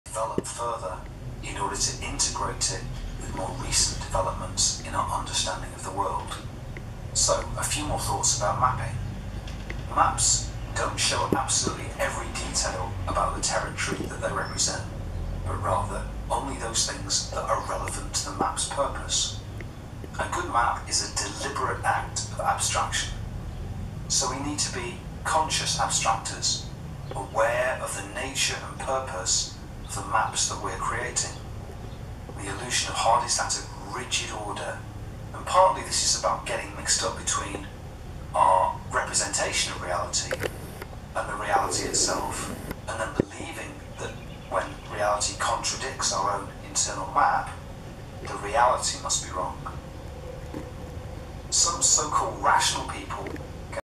Ant carries dead friend and grooms itself
Ants are much more intelligent than most people think, and live complex lives. In many ways ants, bees, and other insects live in societies similar to our own, and each individual has a specialized function to suit the needs of the colony. Who are we to say that they don't also have consciousness, and emotions?